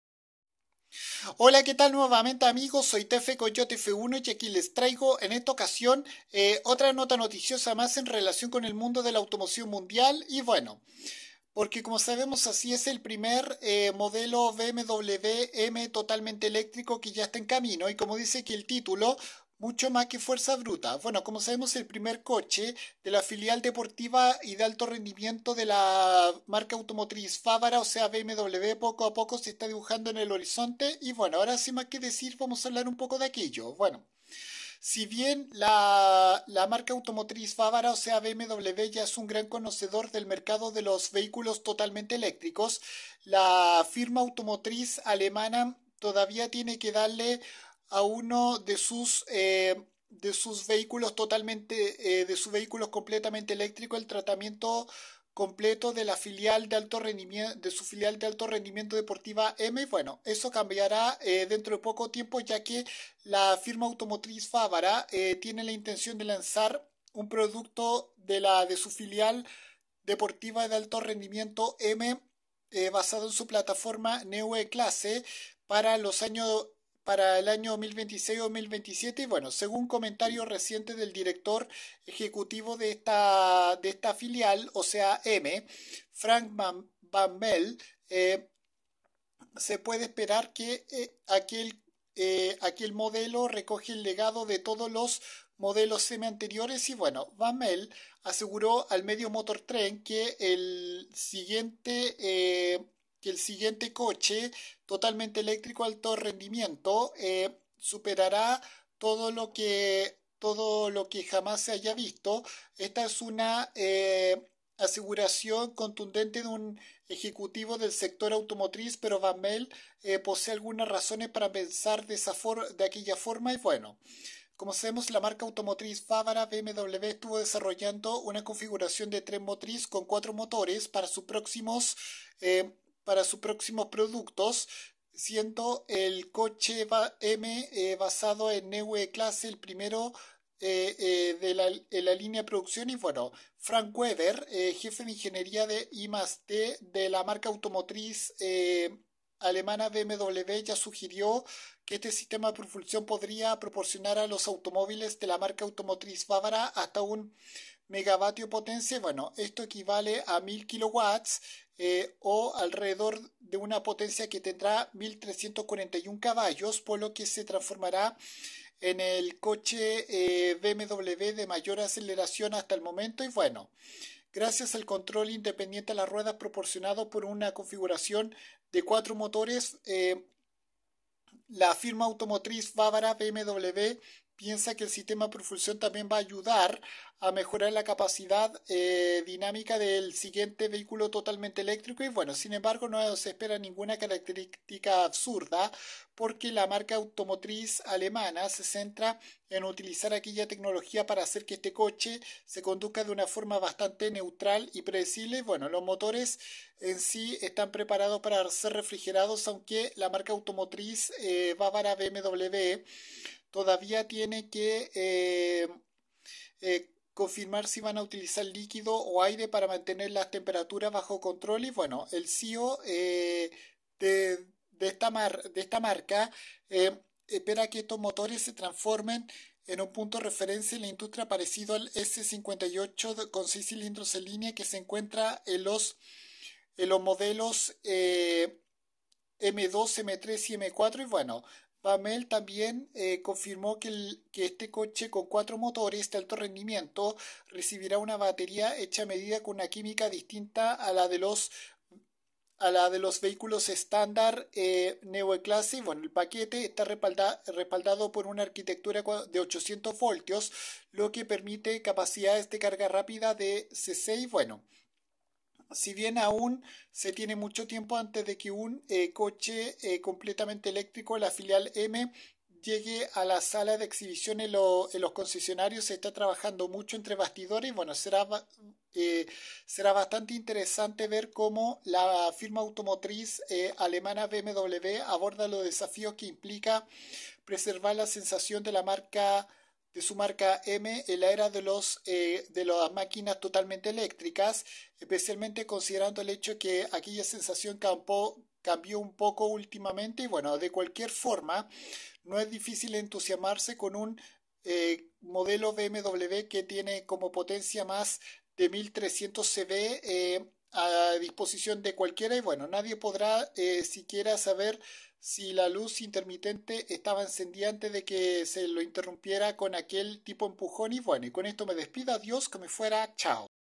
Ya está en camino el 1er modelo de BMW M 100% eléctrico
Ya se está preparando el primer coche totalmente eléctrico de la filial deportiva y de alto rendimiento M de BMW